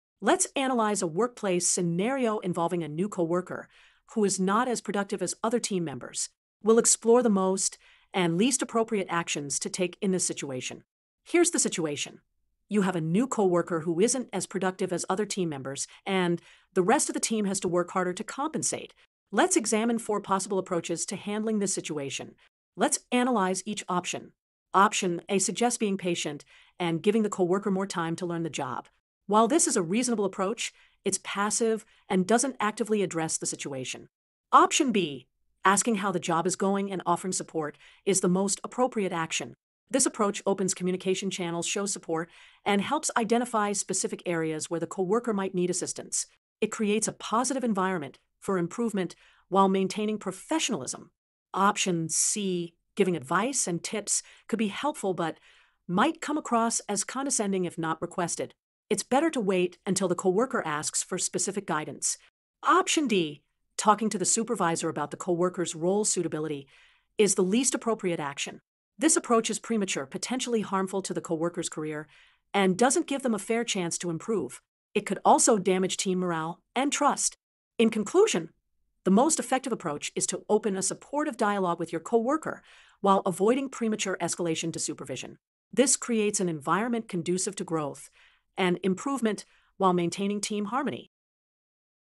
Walmart 2024 - Q3 | Hourly Retail Associate Assessment Walmart | Your newest coworker is not as prod
Your newest coworker is not as productive as other members of your team. You and your coworkers have to work harder to make up for this.What would you be most and least likely to do? (A).Be patient. Give her more time to learn the job so that her productivity will increase. (B).Ask her how the job is going. Let her know you’re available if she has any questions. ©.Give her advice and tips for how to get things done correctly and efficiently. (D).Talk to your supervisor about the situation. Suggest that this might not be the best role for your coworker.

Solution:
Most Likely Action:
(B) Ask her how the job is going. Let her know you’re available if she has any questions.

    This option encourages open communication and offers support, allowing the coworker to feel comfortable seeking help. It is both supportive and constructive, facilitating growth in her productivity.

Least Likely Action:
(D) Talk to your supervisor about the situation. Suggest that this might not be the best role for your coworker.

    This action may be premature and could negatively impact the coworker’s confidence and morale. It does not directly address the issue with constructive support or feedback.

Conclusion:

    Most Likely: (B) Ask her how the job is going; offer your support.
    Least Likely: (D) Talk to your supervisor about her role suitability.